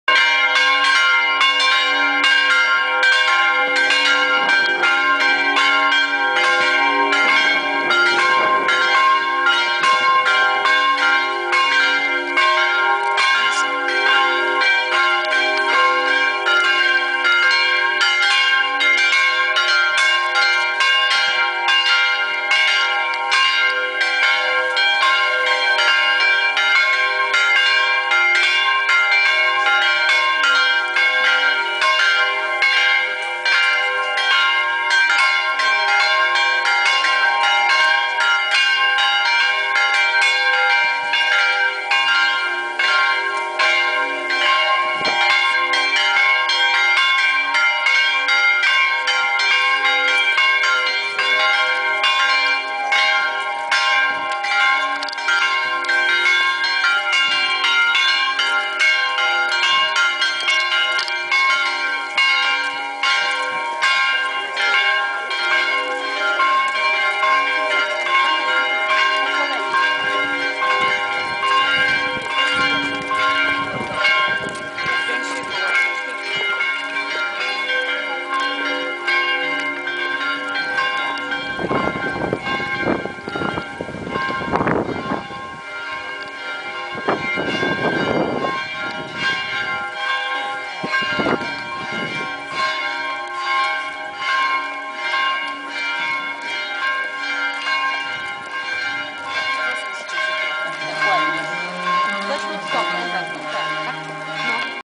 2013 08 25 MBCzestochowskiej 008
Odpust z Rycerzami Kolumba w parafii Matki Boskiej Częstochowskiej w Sosnowcu - Porąbce, 25 sierpnia 2013r., autor filmu: Piotr Dudała